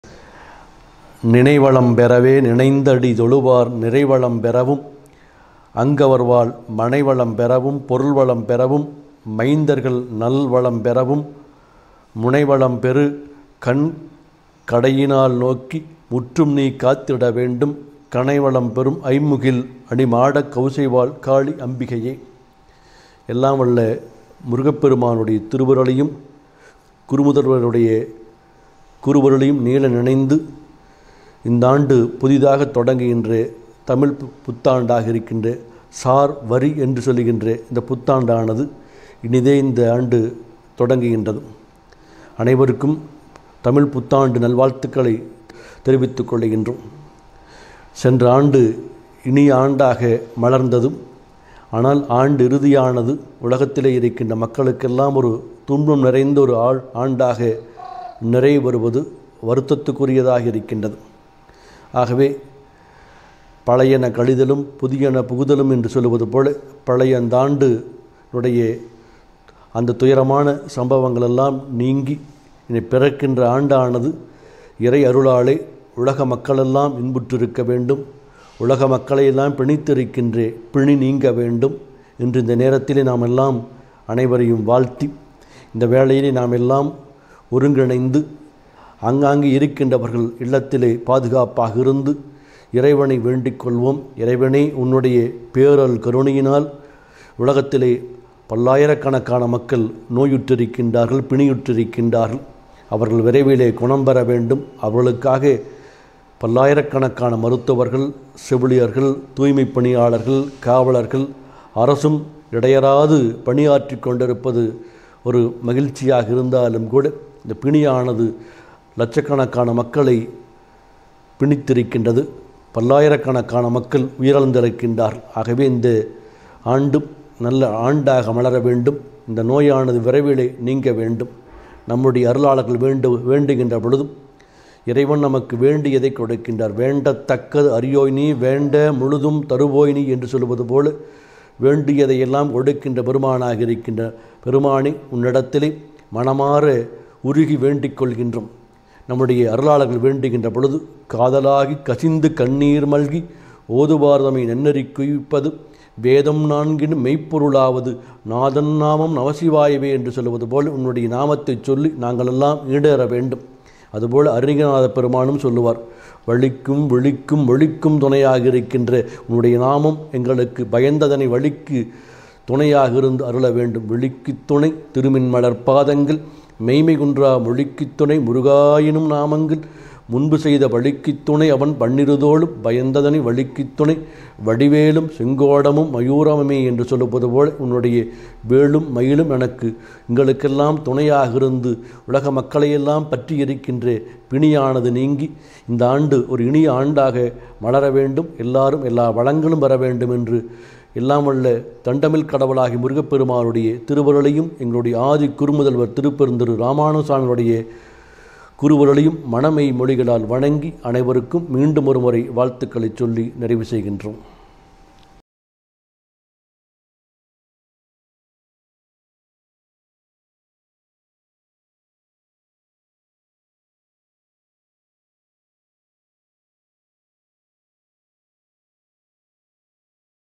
இனிய தமிழ்ப் புத்தாண்டு நல்வாழ்த்துக்கள் | சிரவை ஆதீனம் ஆசியுரை
கோவை, கௌமார மடாலயம், சிரவை ஆதீனம் குருமகா சன்னிதானங்கள் ளின் தமிழ்ப் புத்தாண்டு ஆசியுரை